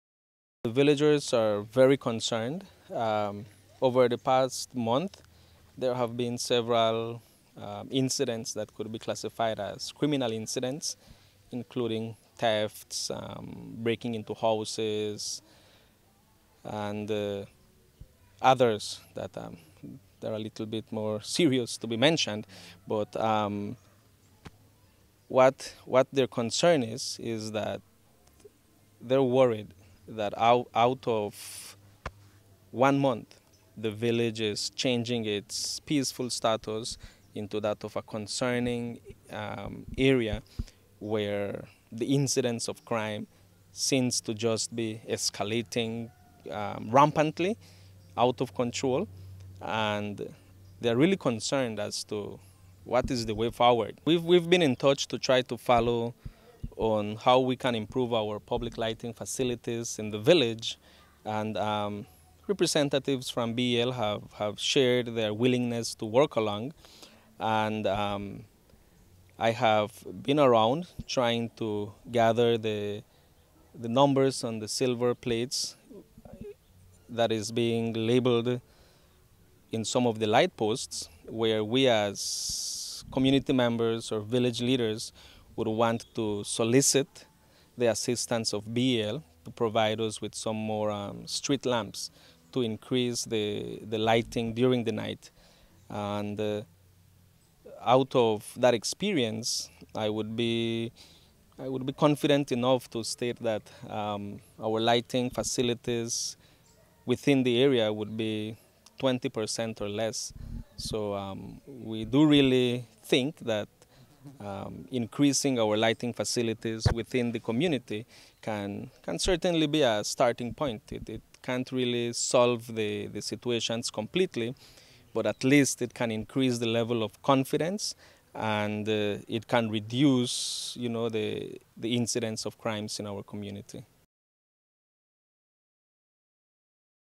Crime Up in Cow Pen; Village Needs More Street Lights
In the south, retired rural development officer Santiago Pop was also murdered over the weekend. The fifty-eight-year-old was chopped to death inside his house sometime between Friday and early Saturday morning. His body was discovered inside his house in Cow Pen Village around six-thirty on Sunday evening. When we spoke with Village Chairman Victor Juarez, he told us that for the past month there have been several crimes committed in the village but the Pop murder has left the community shaken. He says that the escalation in criminal activities is concerning. One of the issues that are repeatedly brought up by villagers is the lack of street lights in the village. According to Chairman Juarez, only about twenty-percent of the village has street lights and the many of the crimes that are being committed are on streets that are poorly lit or that have no light at all.